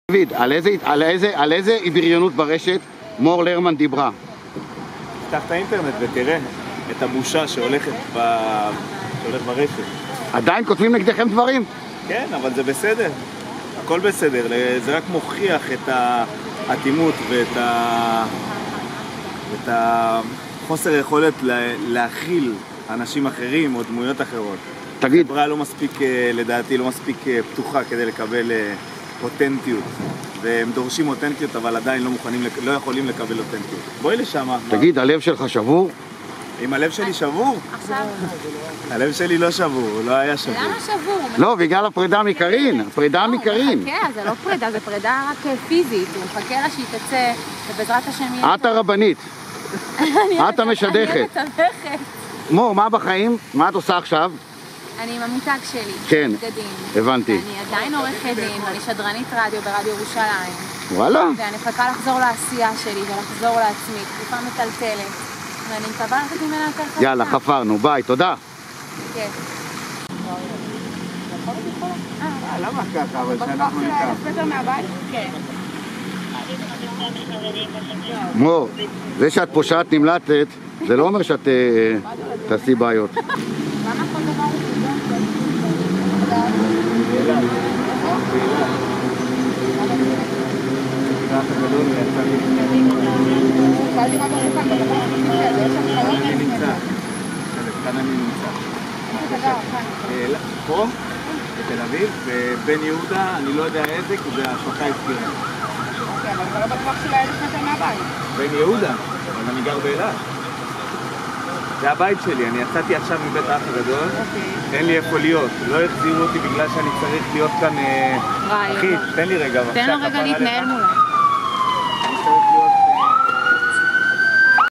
לא סתם פפראצי: שוטרים הפריעו לדייט של מור לרמן ודיוויד דימוס
המודחים הטריים מבית "האח הגדול" קבעו להיפגש ללאנץ' בכיכר דיזנגוף, אבל לרוע מזלם, שוטרים שעברו בסביבה החליטו להפריע למפגש. איזה מזל שמצלמת הפפראצי של "פנאי פלוס" הייתה שם לתעד את הרגע